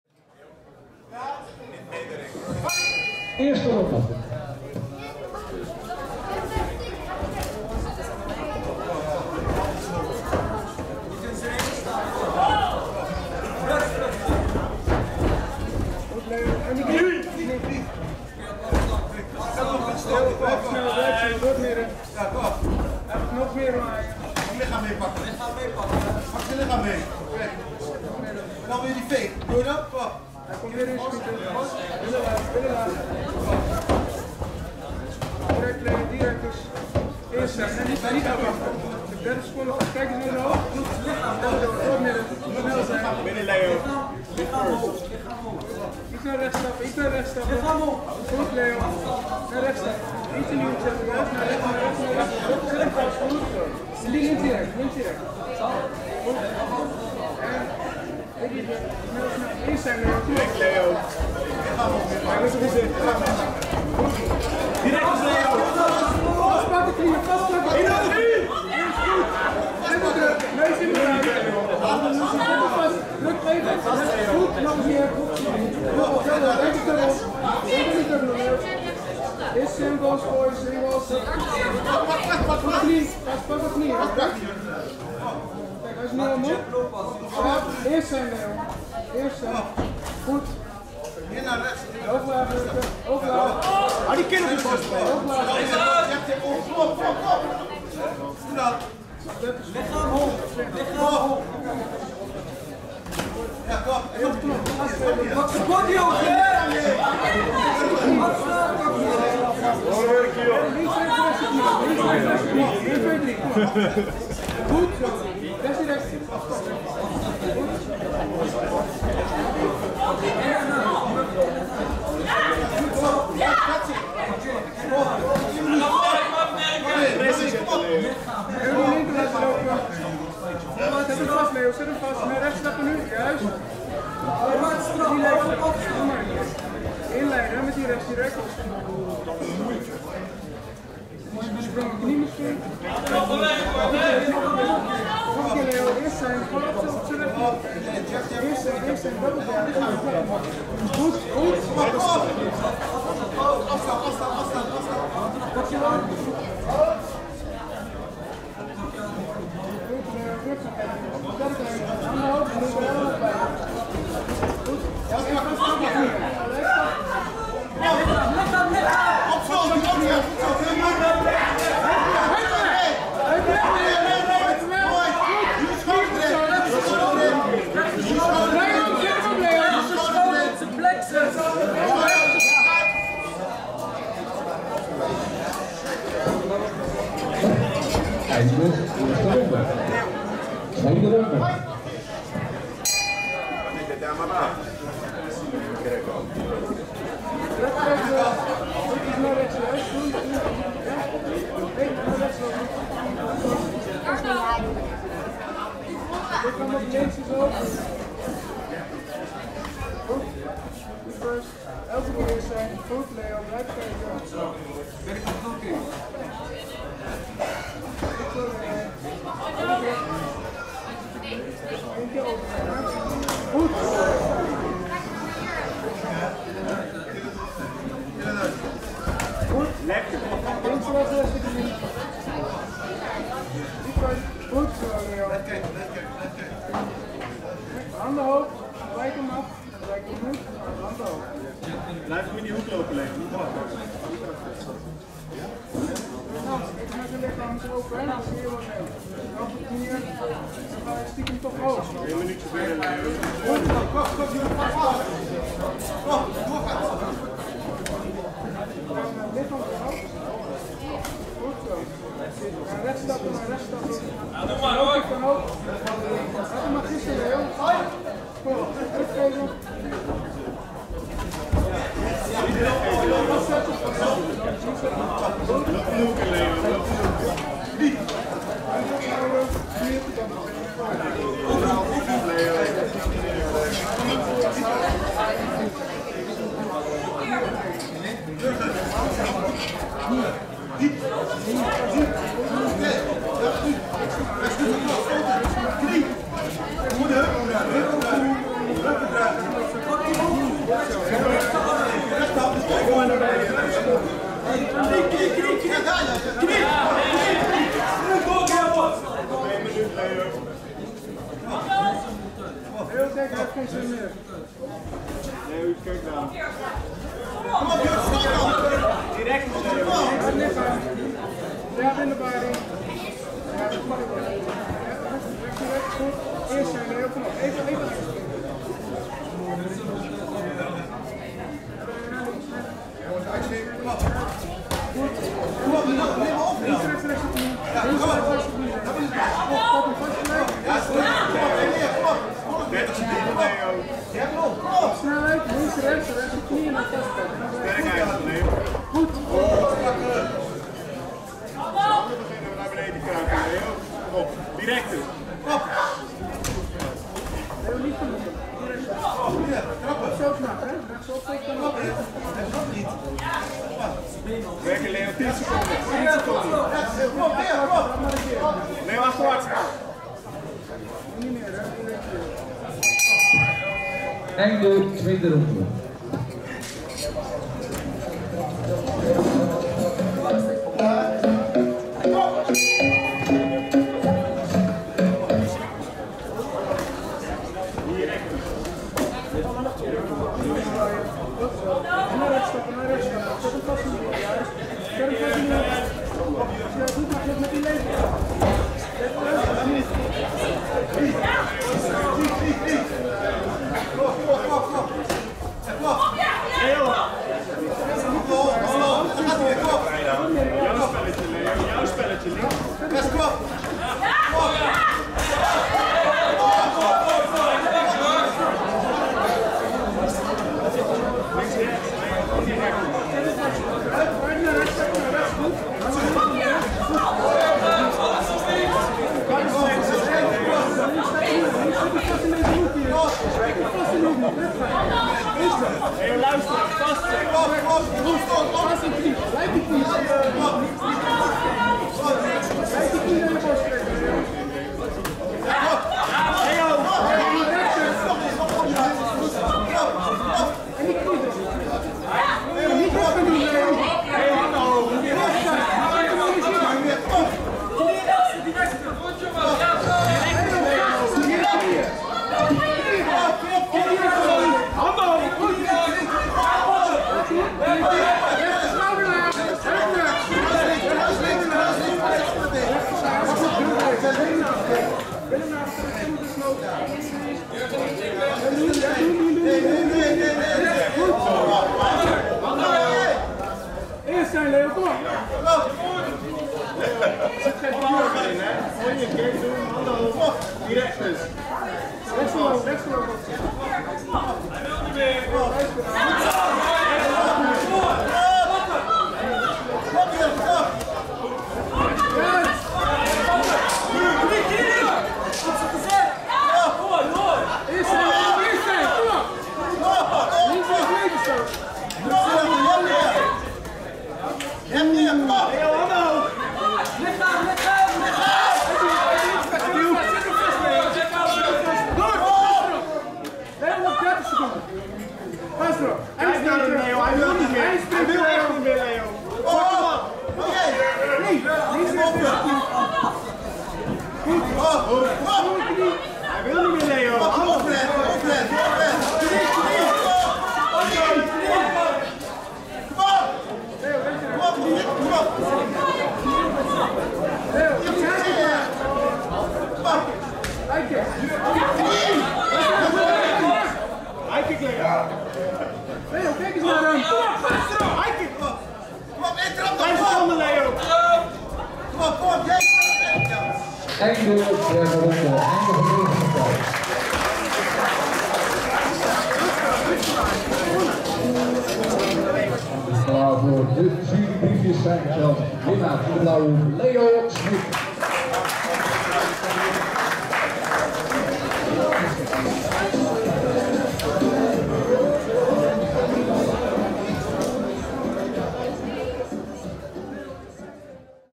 Jurgen van der Voet vs Leo Smit
MMA amateur partij 3x3 minuten
Summer Slamm - Last Fights Before Summer